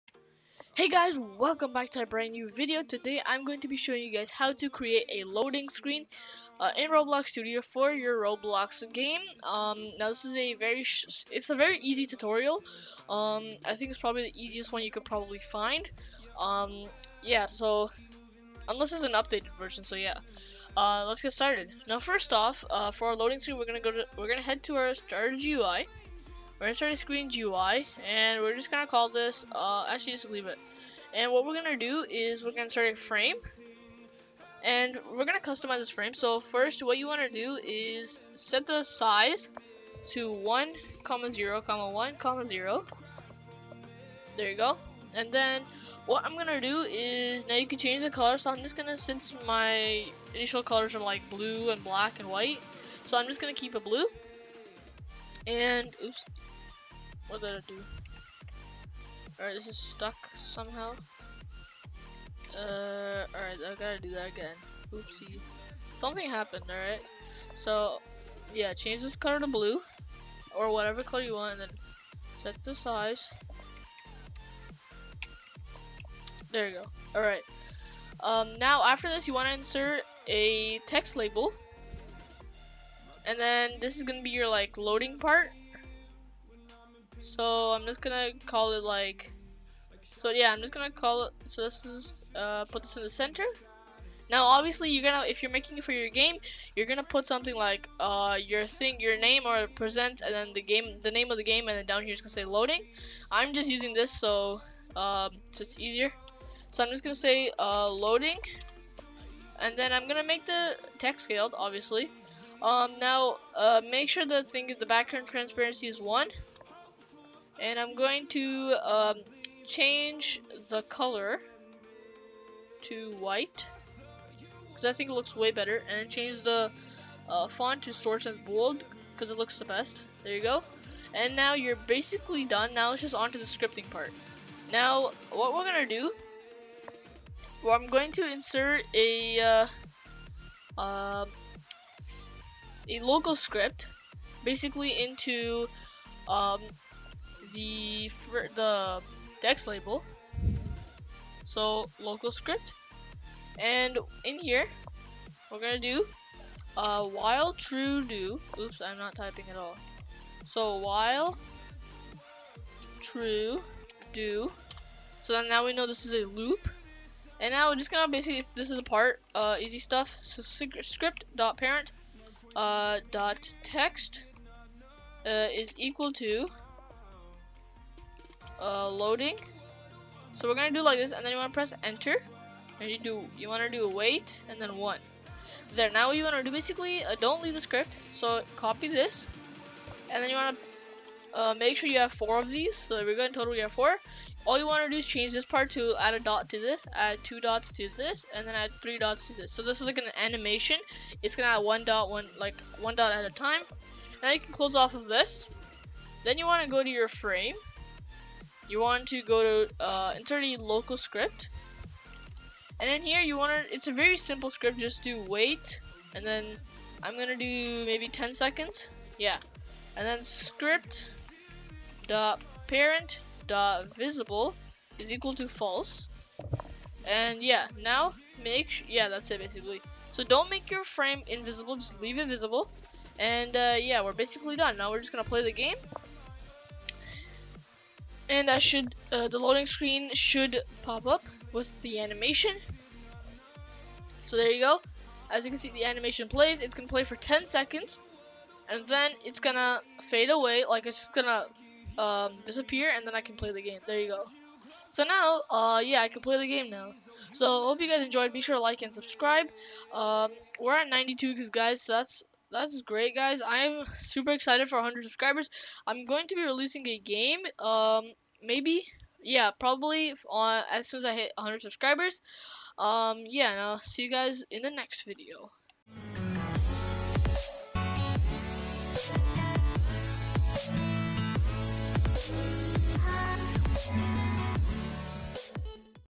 How to make a loading screen in Roblox Studio
In this video I will show you how you can make your own loading screen for your game.